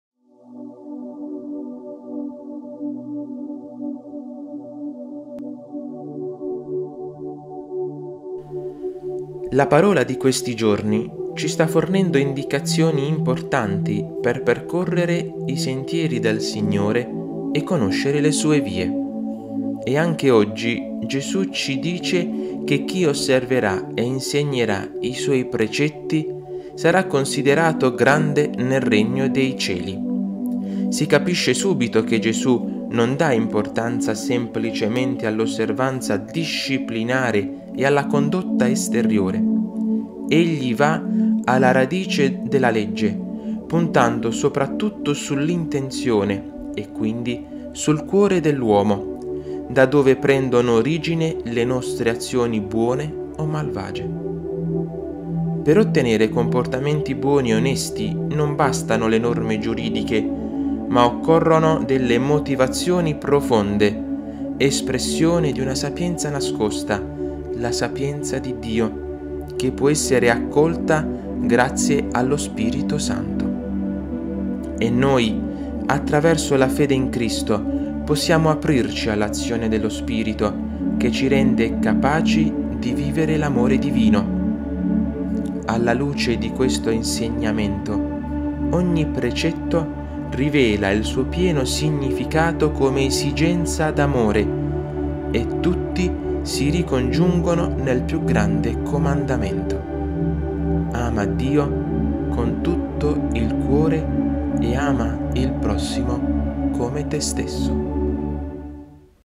🟣 PAROLE DI QUARESIMA ▪ Mercoledì 6 marzo
Mercoledì 6 marzo ▪ III settimana di Quaresima

La Parola di questi giorni ci sta fornendo indicazioni importanti per percorrere i sentieri del Signore e conoscere le sue vie. E, anche oggi, Gesù ci dice che chi osserverà e insegnerà i suoi precetti sarà considerato grande nel Regno dei Cieli.
Si capisce subito che Gesù non dà importanza semplicemente all'osservanza disciplinare e alla condotta esteriore. Egli va alla radice della Legge, puntando soprattutto sull'intenzione e, quindi, sul cuore dell'uomo, da dove prendono origine le nostre azioni buone o malvagie.
Per ottenere comportamenti buoni e onesti non bastano le norme giuridiche, ma occorrono delle motivazioni profonde, espressione di una sapienza nascosta, la Sapienza di Dio, che può essere accolta grazie allo Spirito Santo.
E noi, attraverso la fede in Cristo, possiamo aprirci all'azione dello Spirito, che ci rende capaci di vivere l'amore divino.
Alla luce di questo insegnamento, ogni precetto rivela il suo pieno significato come esigenza d'amore, e tutti si ricongiungono nel più grande comandamento: ama Dio con tutto il cuore e ama il prossimo come te stesso.